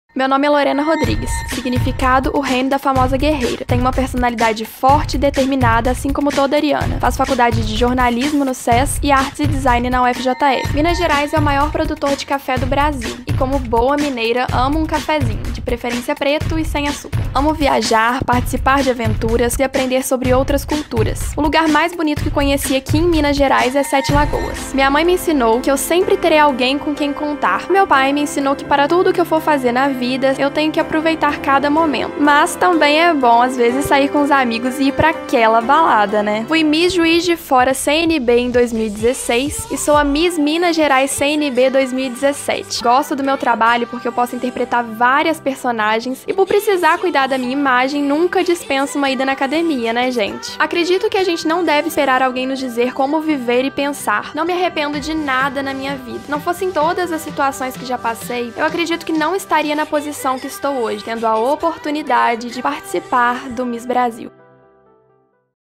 Miss Brasil Mundo 2017 - Vídeo de Apresentação - Miss Minas Gerais CNB
Lorena Rodrigues - Miss Minas Gerais CNB 2017 estará no Miss Brasil Mundo 2017 que ocorrerá no dia 12 de Agosto diretamente do Hotel do Bosque em Angra dos Reis - RJ, com Transmissão da Rede Brasil.